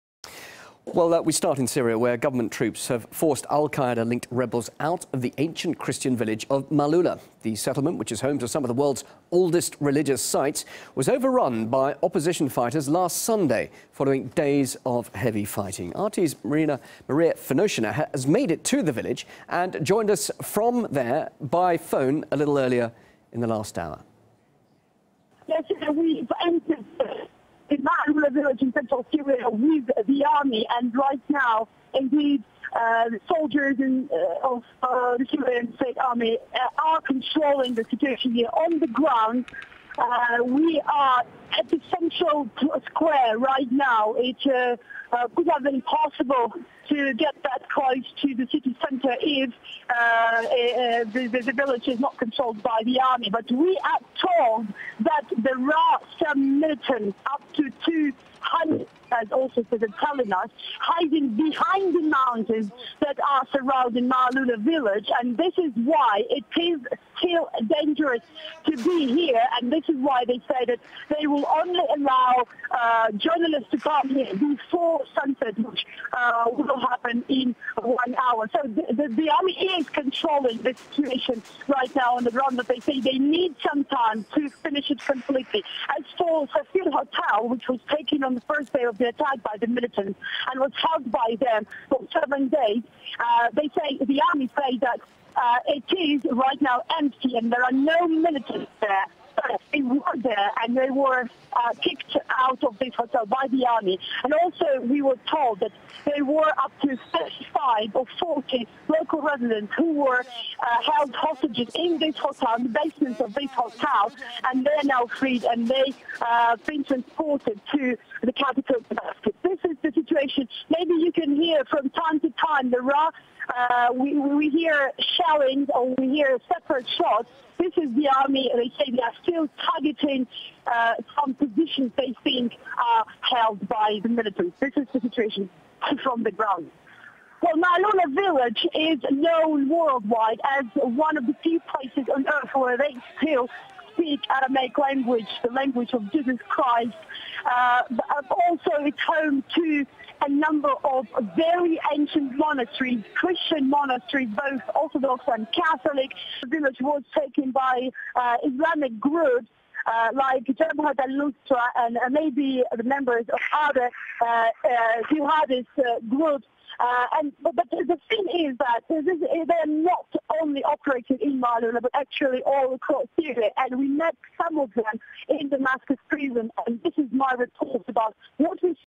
Laporan RT LIVE pertempuran jihad Tentera Syria di kampung Kristian KUNO purba
MALA'yuMADA'yu | Manasseh@1WORLDCommunity
End Of the History, Not End Of the WORLD!

Frontline: Dramatic report as Syria Army battles jihadists in ancient Christian village

The Syrian army is battling small groups of rebels who remain in the ancient Christian village of Maaloula. Opposition fighters linked to Al-Qaeda have been in control of the area for almost a week - reportedly looting religious sites and forcing residents to convert to Islam at gunpoint.

RT's Maria Finoshina made it to the village following reports that government troops have re-captured it - but found pockets of resistance remain.

RT live report: Syrian Army battles jihadists in ancient Christian village

Sporadic fighting continues after Syrian government troops have regained control of the village of Maaloula, a center of Christianity in the region. RT's correspondent reports from the village, amid the shooting and fighter jets overhead.

Despite the battle, RT's crew made to the central square of Maaloula, the first time since they arrived in Syria on Saturday.

From there they headed towards Mar Takla (St.Thakla) - one of the two oldest surviving monasteries in Syria -- on the edge of Maaloula.

On the way they were caught in cross fire during which the camera-man's assistant received a mild injury.

"We were shooting a package. It was already dark and suddenly militants started to fire. We had to hide between houses. The situation was getting worse and worse. As a result, Kirill's forehead was slashed by a ricochet," RT's camera man said.

On Wednesday, Syrian government troops killed eight rebels and injured 20 others.

Inside the village fire was mainly targeting the mountaintop Safir Hotel, which the Al-Nusra Front militants seized last Wednesday. There were no militants inside, the army said, but some 200 jihadists are still hiding in the mountains that surround Maaloula.

"It's still dangerous to be here, and this is why they [army] allow journalists here only before sunset," Maria Finoshina reported. "According to the army there were local residents who were kept hostage in the basement of this hotel, but they are also free now," she added.

On Saturday pro-Assad forces managed to regain control for a short period of time, however by in the early hours of Sunday morning militants had retaken the village.

That occasion also saw the RT crew come perilously close to the fighting.

"It's hard to believe that what used to be one of the most significant shrines of the Christian community all over the world is a battlefield now," Finoshina reported from the village on Saturday, her voice occasionally drowned out by the sound of gunfire.

Maaloula -- a mountain village of 2,000 residents, 60km northeast of Damascus - is a major pilgrimage destination for Christians from around the world.

It's home to some of the most ancient Orthodox Christian relics and is also one of the very few places in the world where people still speak Aramaic, a biblical-era language that Jesus is believed to have spoken.

Apart from being an ancient sanctuary on a UNESCO list of proposed World Heritage sites, Maaloula is also strategically important, situated on a highway between Damascus and Homs. The rebels have already established bases in the west and south of the capital. The village in the northeast would significantly increase rebel pressure on Damascus.

GOD Almighty Say in the HOLY 'QURAN: 'No Reward Do I Ask Of You For It: MY REWARD IS ONLY FROM THE LORD OF THE UNIVERSE'. (QS 26:109)